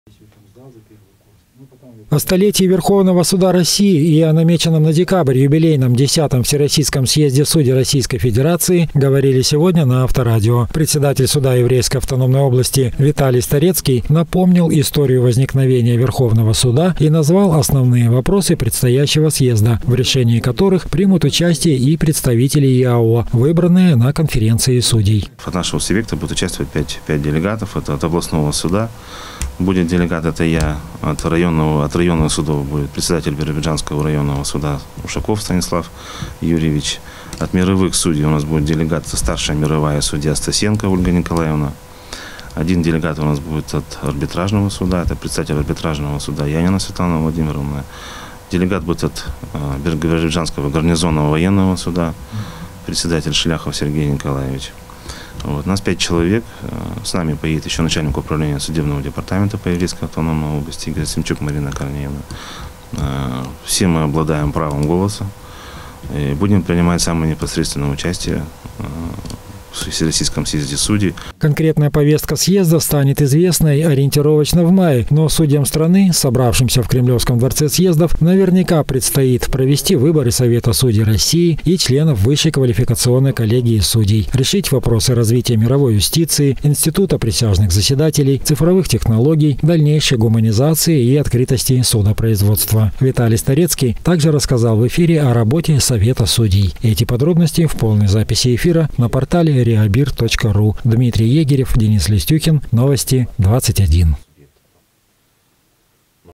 О вековой дате Верховного суда России и 10-м съезде судей рассказали слушателям Авторадио
О столетии Верховного Суда России и о намеченном на декабрь юбилейном 10-м Всероссийском съезде судей Российской Федерации говорили на Авторадио. Председатель суда Еврейской автономной области Виталий Старецкий напомнил историю возникновения Верховного суда и назвал основные вопросы предстоящего съезда, в решении которых примут участие представители ЕАО, выбранные на конференции судей.